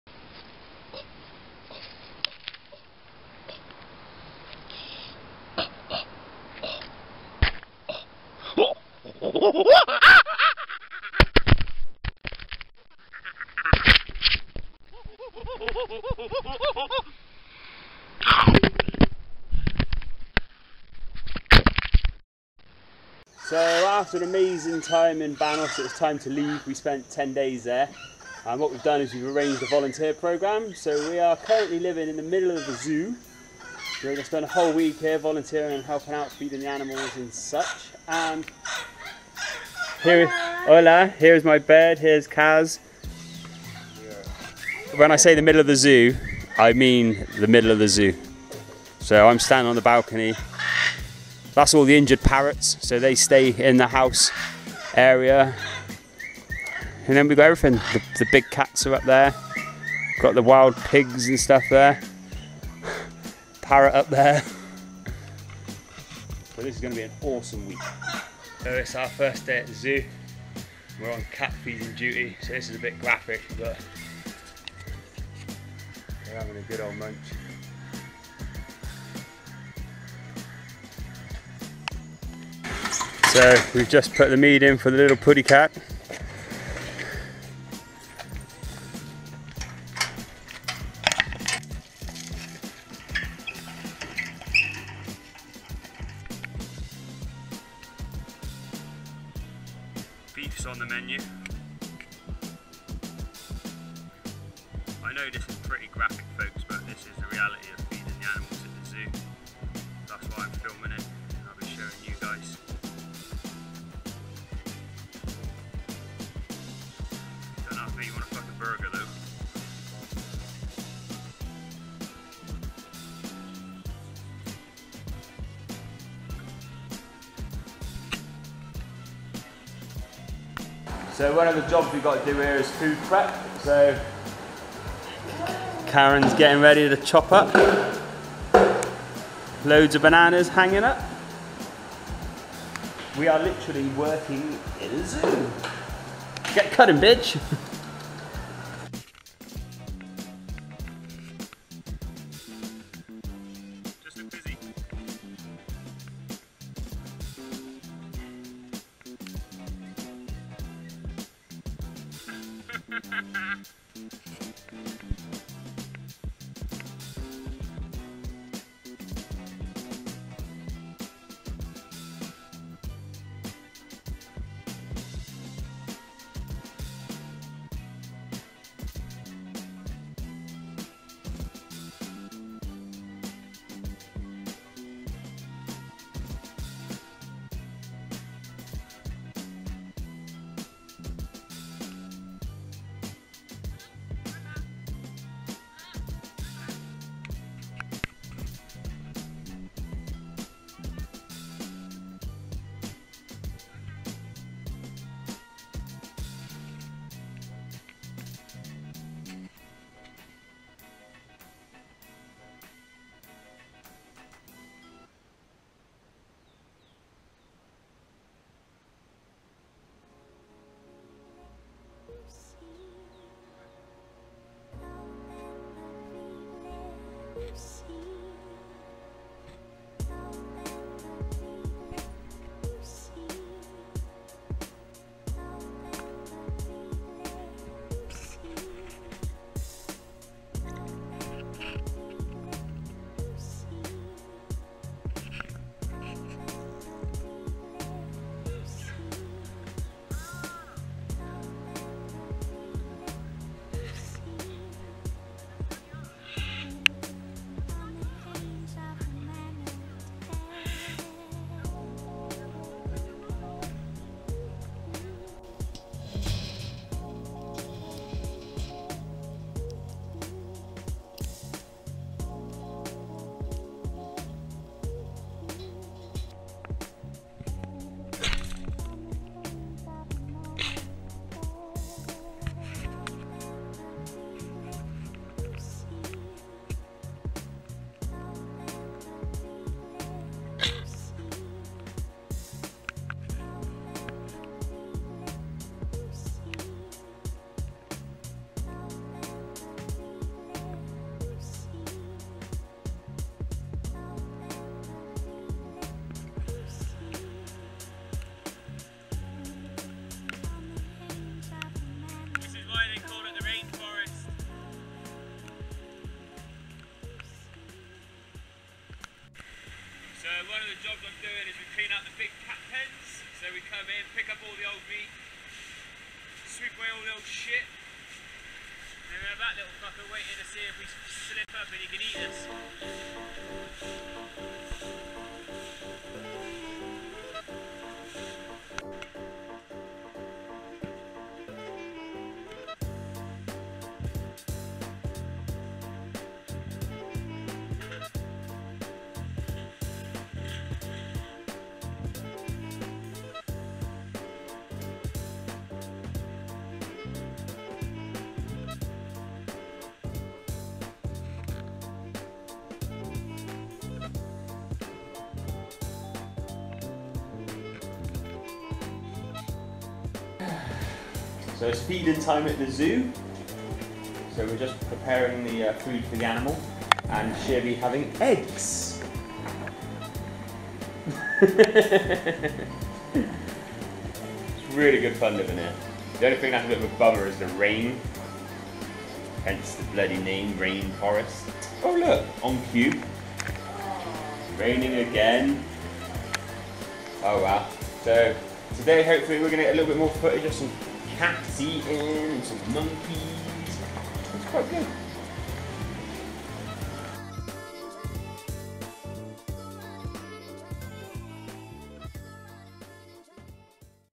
TRAVEL MONKEYS ECUADOR PUYO VOLUNTERRING AT THE ZOO
PT 1 OF 3 OF ARE AMAZING WEEK AT TARQUI ZOO NR PUYO, LOOKING AFTER AND FEEDING THE ANIMALS